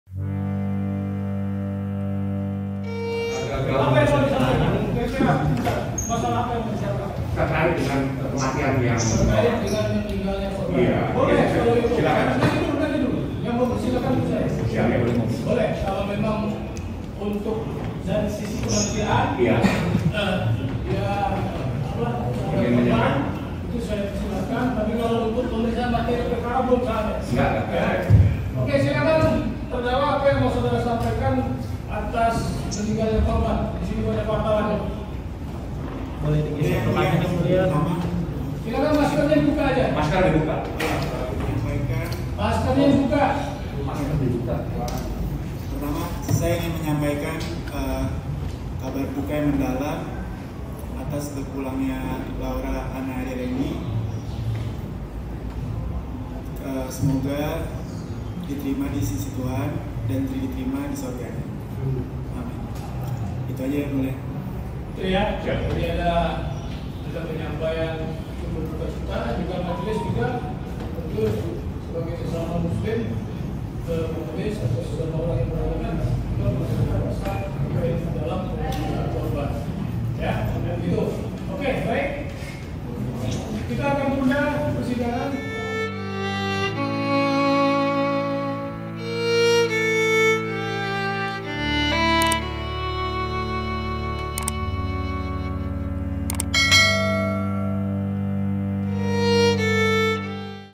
Gaga Muhammad Sampaikan Duka Cita atas Meninggalnya Laura Anna
Selebgram Laura Anna meninggal dunia pada Rabu (15/12/2021) Pagi.

Melalui video, Gaga menyampaikan duka cita atas kepergian mantan kekasihnya itu. Gaga berharap Laura bisa diterima di sisi Tuhan.

Sebagai informasi, sebelum meninggal dunia, Laura Anna sendiri telah berjuang hidup setelah menderita cervical vertebrae dislocation atau dislokasi tulang leher selama hampir tiga tahun.

Akibat penyakit itu, Laura Anna mengalami kelumpuhan. Laura Anna mengalami kelumpuhan pascakecelakaan bersama mantan kekasihnya, Gaga Muhammad, pada 8 Desember 2019 lalu.

Saat ini, Gaga Muhammad sudah berstatus terdakwa dan kasusnya tengah disidangkan di Pengadilan Negeri Jakarta Timur.

Video Jurnalis: Pramulya Sadewa
Video Editor: Yohana Indah Nur Ratri
Produser: Adisty Safitri